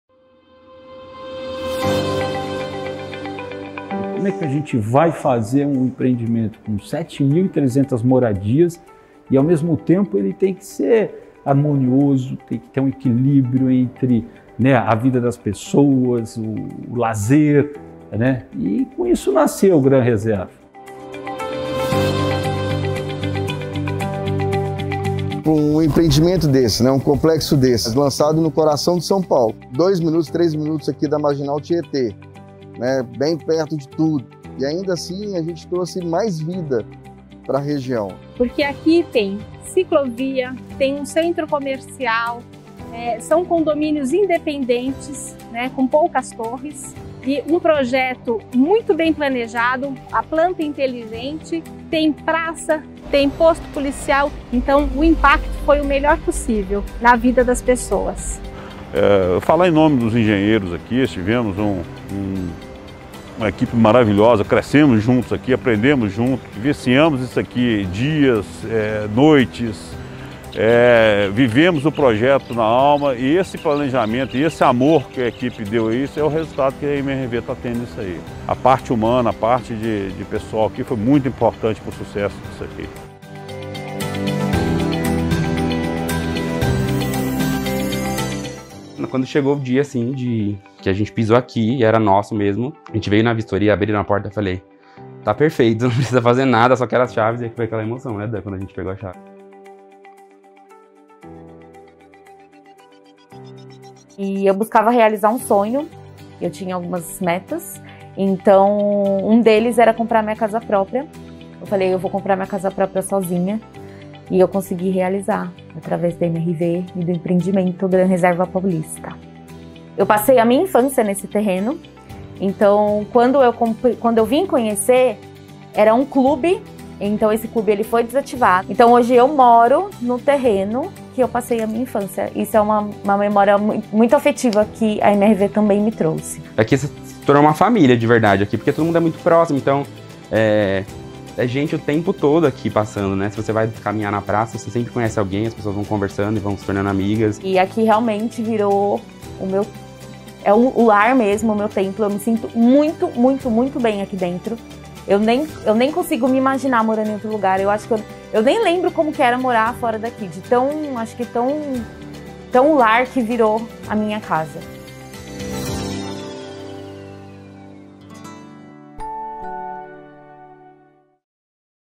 Como é viver no Grand Reserva Paulista, da MRV?
MRV, construindo sonhos que transformam o mundo.